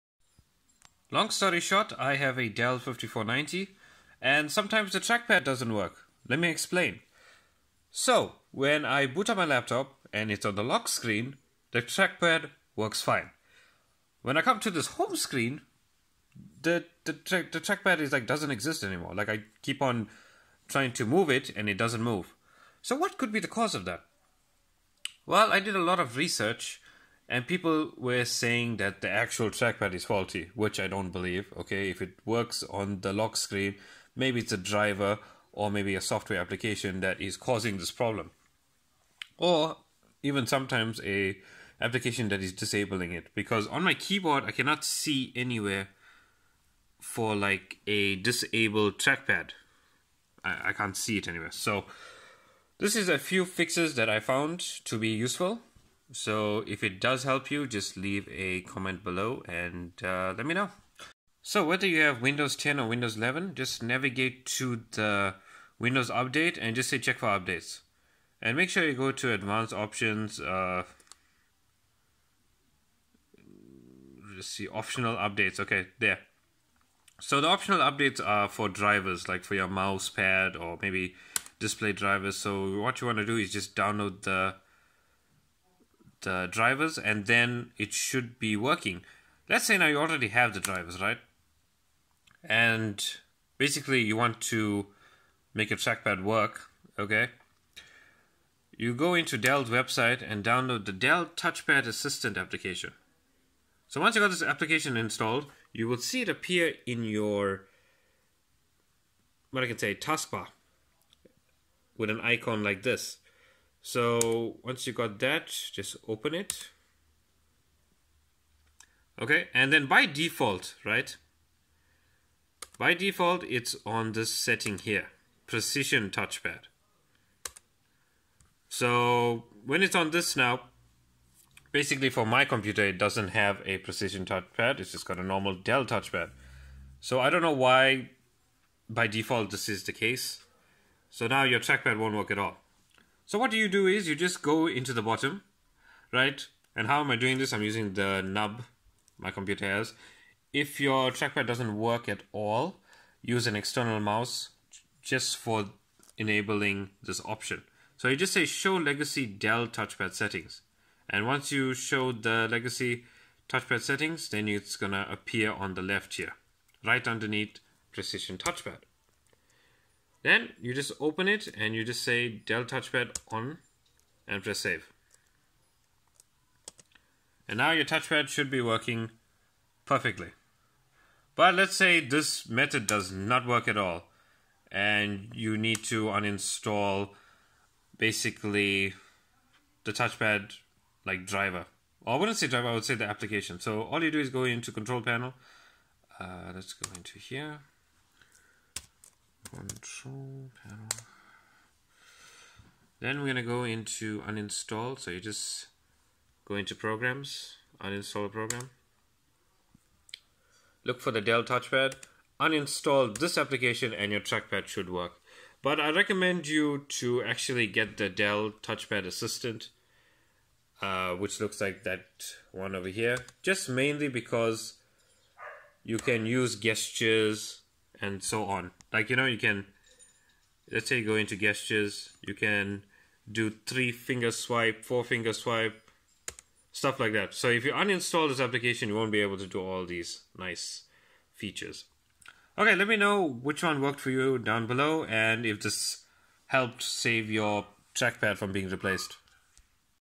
Dell trackpad touch pad not working (Resolved)
Check Your drivers in Device Manager
Install them either on Dells website or through Windows Update
Then get the application for the touch pad and enable the touch bad settings
If you cannot find it after installing it just use the search bar to look for the app and last resort is uninstalling the software through control panel